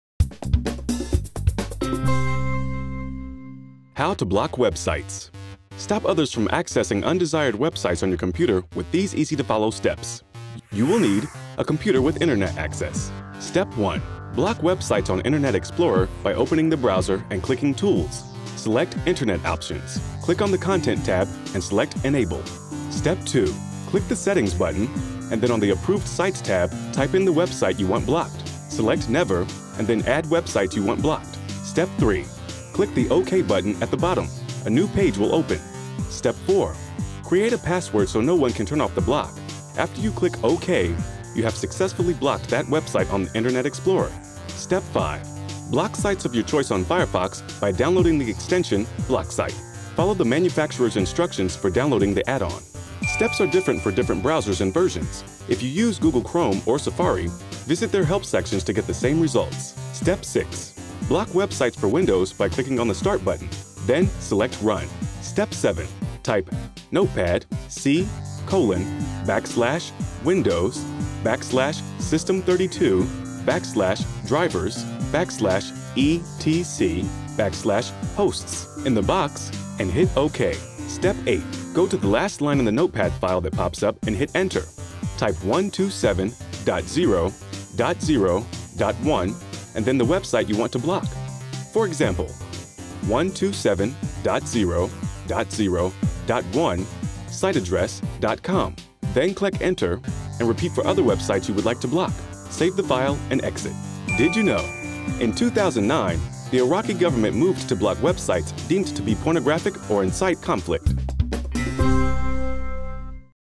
How to Block Web Sites
Stop others from accessing undesired web sites on your computer with these easy to follow steps.

Step 1: Block websites
Block web sites on Internet Explorer by opening the browser and clicking Tools. Select Internet Options. Click on the Content tab and select Enable.

Step 2: Select Never
Click the Settings button, and then on the Approved Sites tab and type in the web site you want blocked. Select Never, and then add web sites you want blocked.

Step 3: Click OK
Click the OK button at the bottom. A new page will open.

Step 4: Create a password
Create a password so no one can turn off the block. After you click OK, you have successfully blocked that web site on Internet Explorer.

Step 5: Block with Firefox
Block sites of your choice on Firefox by downloading the extension BlockSite. Follow the manufacturer's instructions for downloading the add-on.

Tip
Steps are different for different browsers and versions. If you use Google Chrome, or Safari, visit their help sections to get the same results.

Step 6: Block with Windows
Block web sites for Windows by clicking on the Start button. Then select Run.

Step 7: Type in the box
Type "notepad c:WINDOWSsystem32driversetchosts" in the box and hit OK.

Step 8: Type site address
Go to the last line in the notepad file that pops up and hit Enter. Type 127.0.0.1 and then the web site you want to block. For example: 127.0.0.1 siteaddress.com. Then click Enter and repeat for other web sites you would like to block. Save the file and exit.

In 2009, the Iraqi government moved to block web sites deemed to be pornographic or incite conflict.